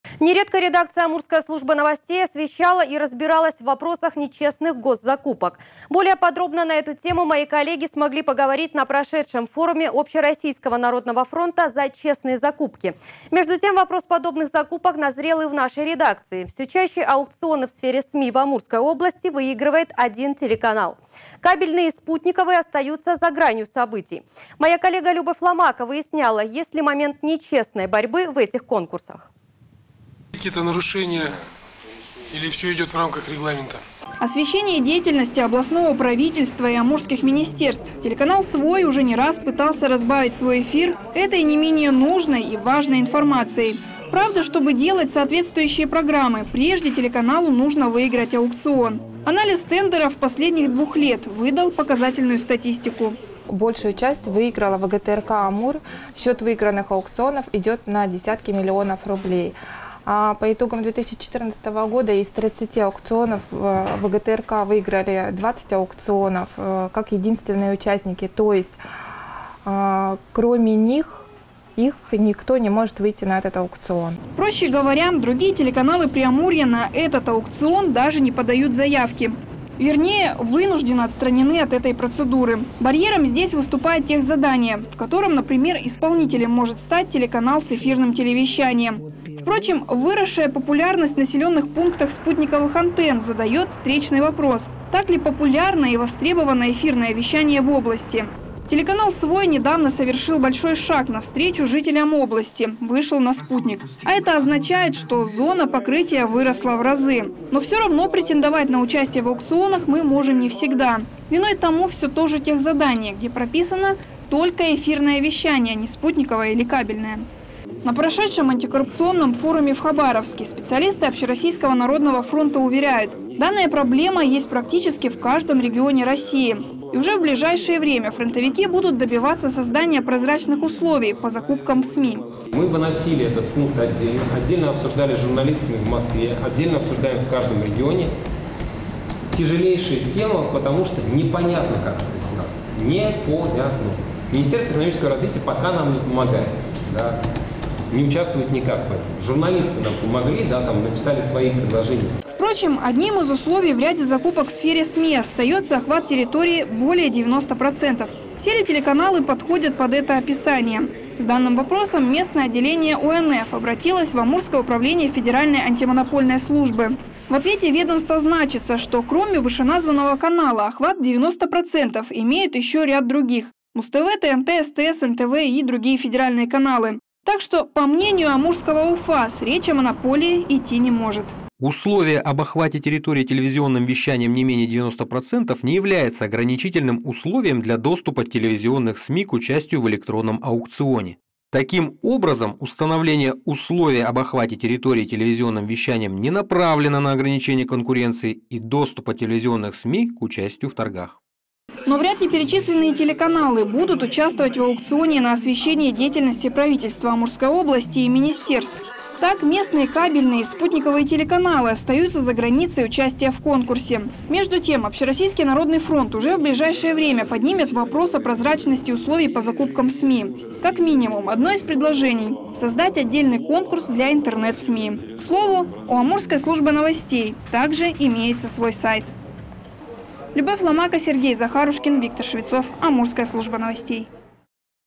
По-честному
Нередко редакция Амурской службы новостей освещала и разбиралась в вопросах нечестных госзакупок. ​Более подробно на эту тему мои коллеги смогли поговорить на прошедшем форуме Общероссийского народного фронта "За честные закупки". ​Между тем, вопрос подобных закупок назрел и в нашей редакции. Все чаще аукционы в сфере СМИ в Амурской области выигрывает один телеканал. Кабельные и спутниковые остаются за гранью событий. Моя коллега Любовь Ломако выясняла, есть ли момент НЕчестной борьбы в этих конкурсах.